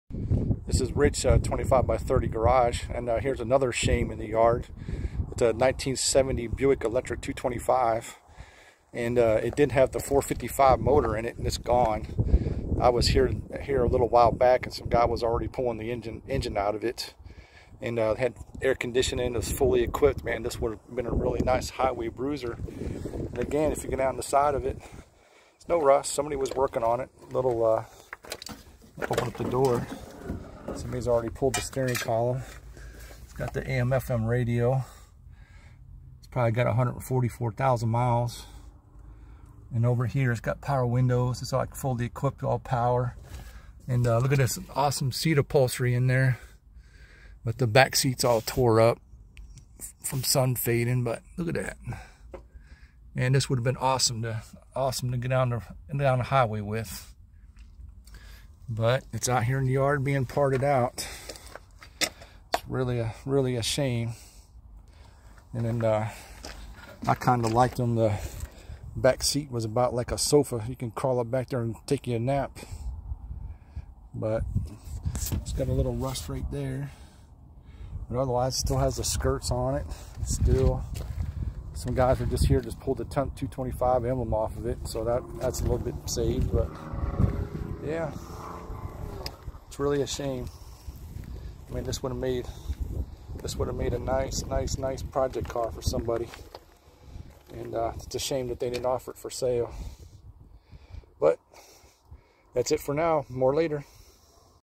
1970 Buick Electra 225
A real shame this is in the yard. It had a 455 motor but it already gone. Check out the interior cloth.